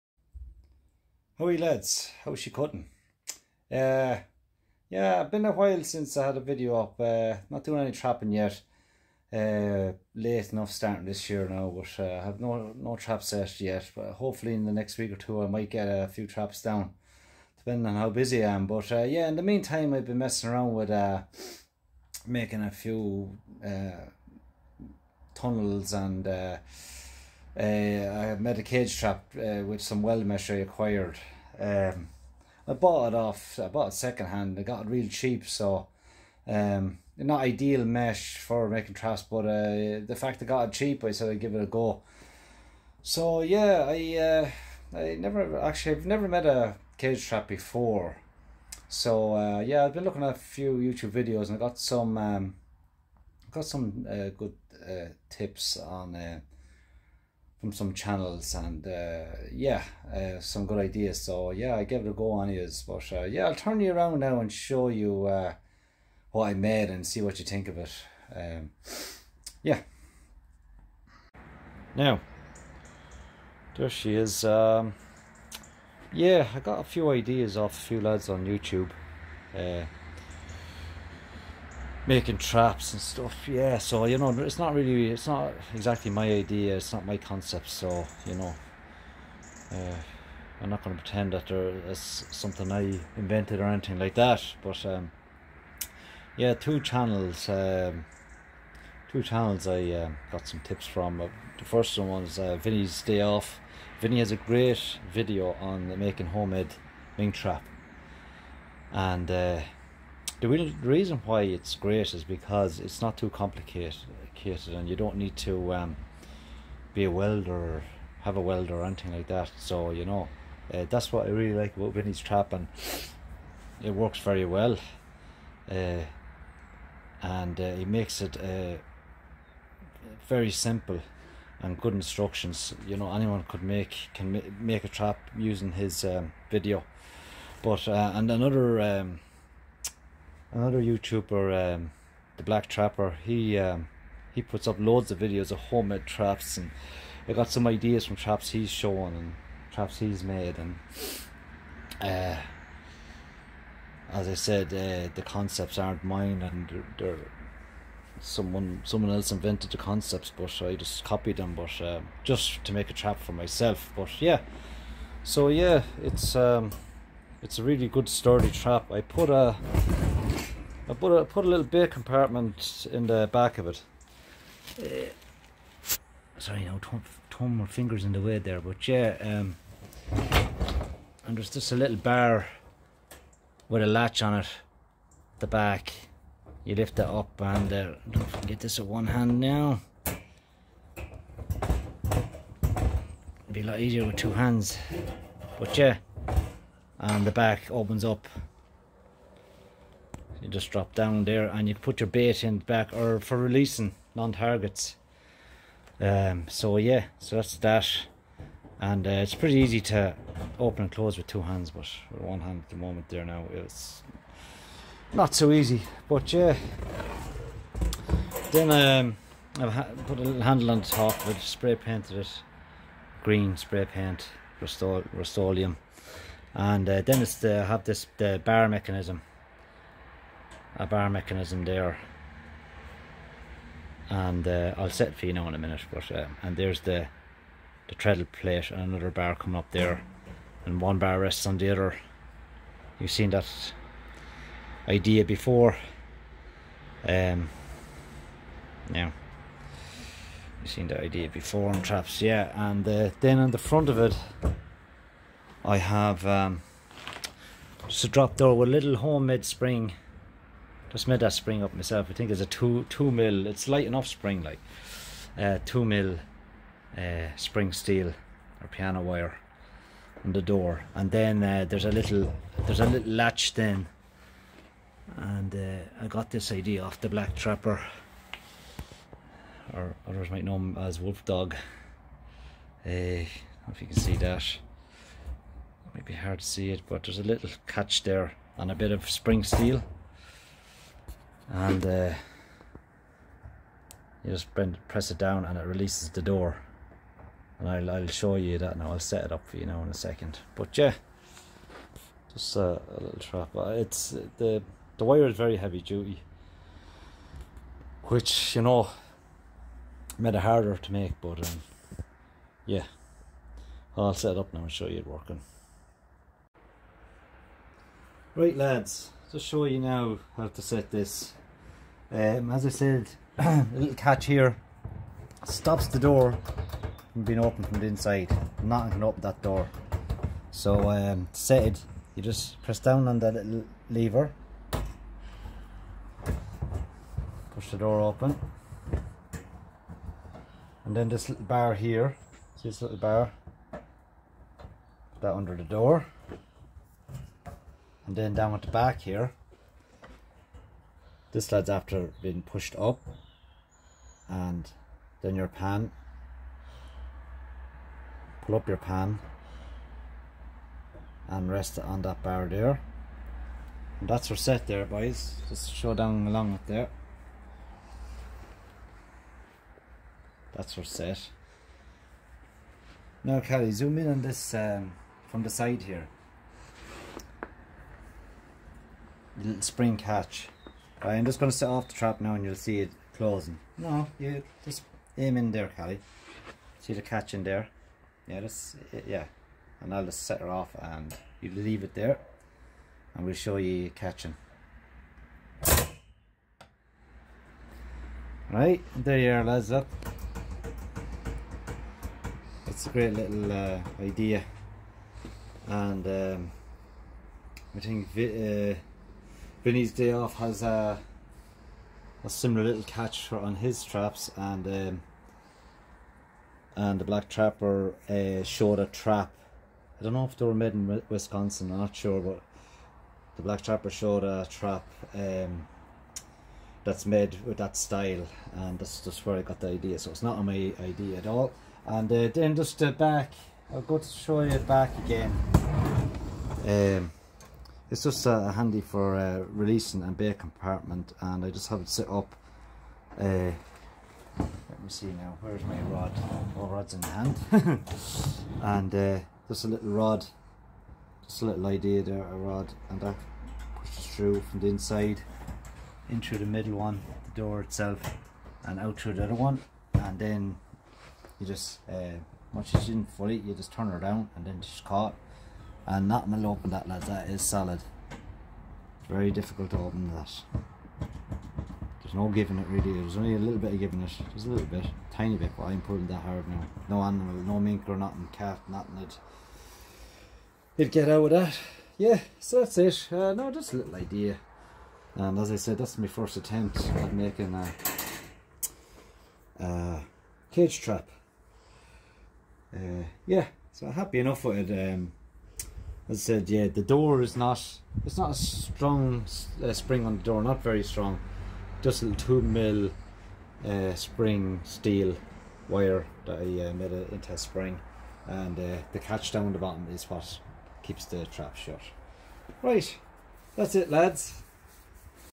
Homemade Cage Trap
Tried my hand at making a cage trap out of some weldmesh I got on the cheap.

 Got some great tips on trap building, from the channels below.